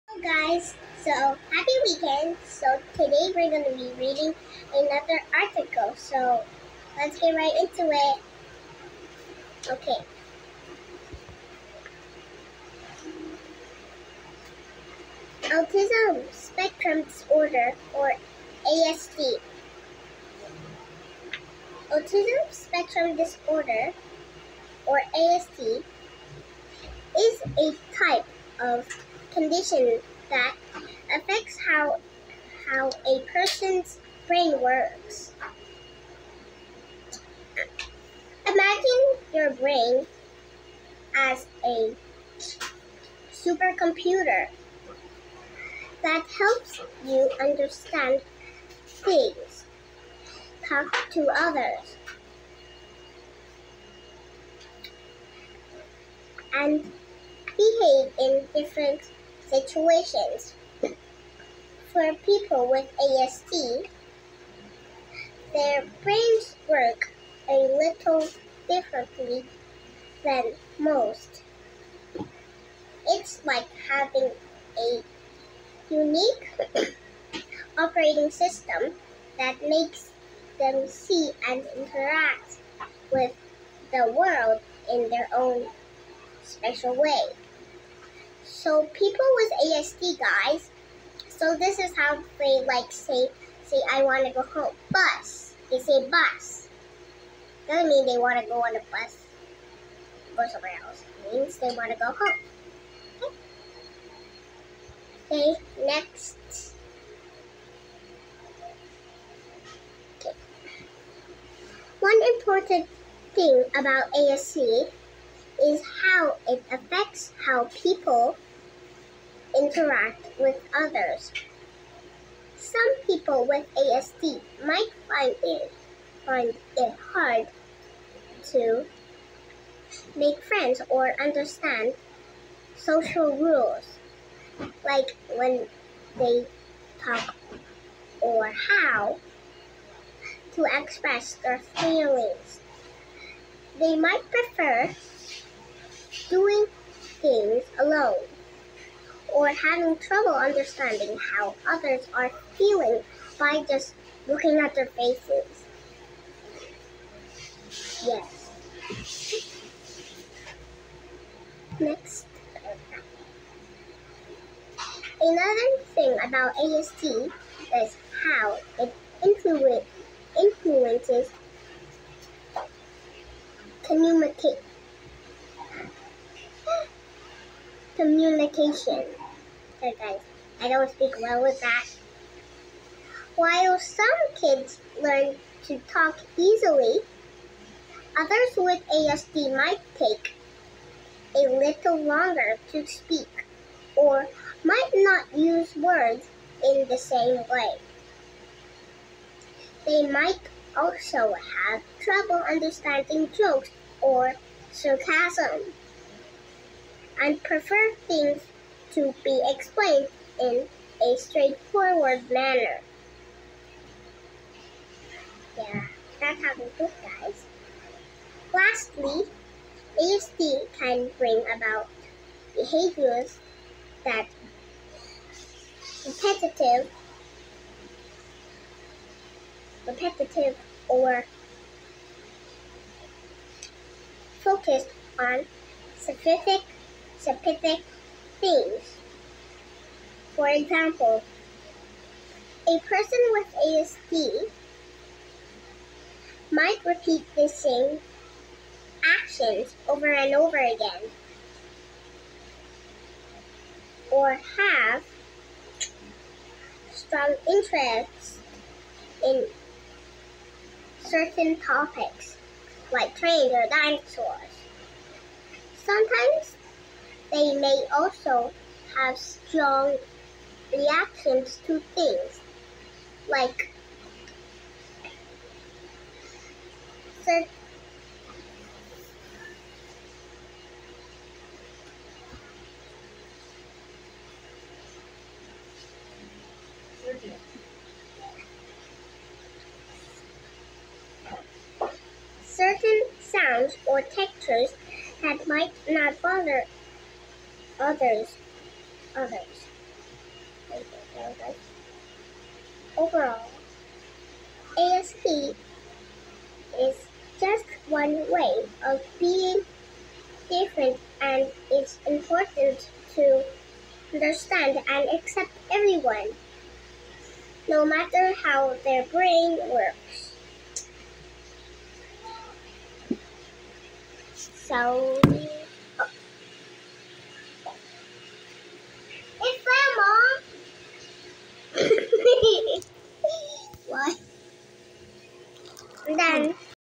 AUTISM SPECTRUM DISORDER
She is just five years old but is already aware of this condition. It's important for kids to know about ASD. Kids are curious about something they see that is unusual to them. Educating them will help them understand certain things that they don't usually see or experience.

SCRIPT:

Autism Spectrum Disorder (ASD)

Autism Spectrum Disorder (ASD) is a type of condition that affects how a person's brain works. Imagine your brain as a supercomputer that helps you understand things, talk to others, and behave in different situations. For people with ASD, their brains work a little differently than most. It's like having a unique operating system that makes them see and interact with the world in their own special way.

One important thing about ASD is how it affects how people interact with others. Some people with ASD might find it hard to make friends or understand social rules like when to talk or how to express their feelings. They might prefer doing things alone or have trouble understanding how others are feeling just by looking at their faces.

Another thing about ASD is how it influences communication. While some kids learn to talk easily, others with ASD might take a little longer to speak or might not use words in the same way. They might also have trouble understanding jokes or sarcasm and prefer things to be explained in a straightforward manner.

Lastly, ASD can bring about behaviors that are repetitive or focused on specific things. For example, a person with ASD might repeat the same actions over and over again or have strong interests in certain topics, like trains or dinosaurs. Sometimes, they may also have strong reactions to things like certain sounds or textures that might not bother others. Overall, ASD is just one way of being different, and it's important to understand and accept everyone, no matter how their brain works.